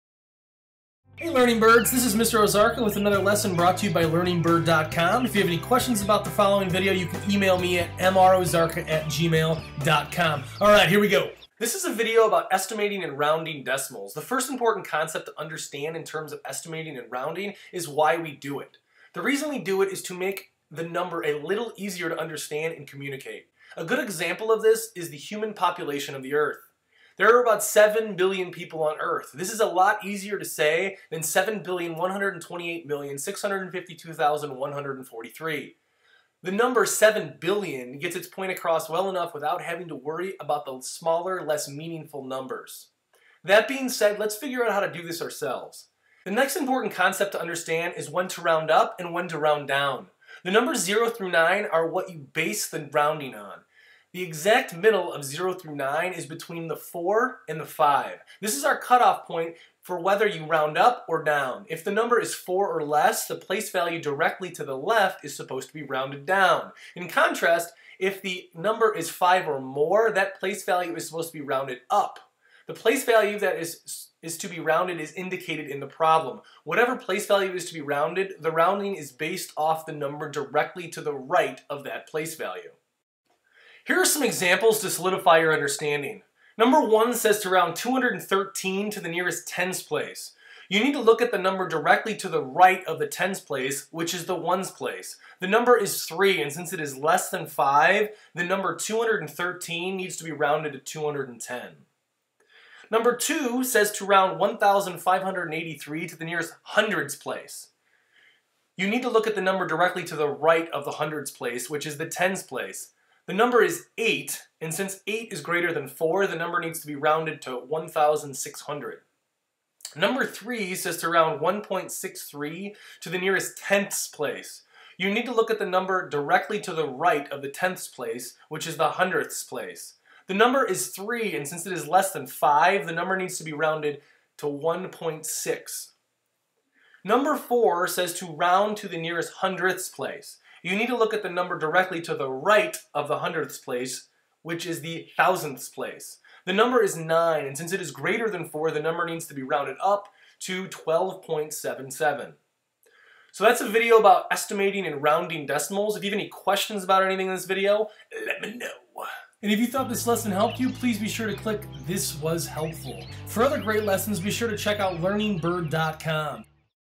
Estimating and Rounding Decimals
This is a brief tutorial on estimating and rounding decimals.

Upon watching this video, you should be able to answer the following questions:

1. Round 3.26 to the nearest tenths place.
2. Round 24.884 to the nearest hundredths place.
3. Round 127.5 to the nearest ones place.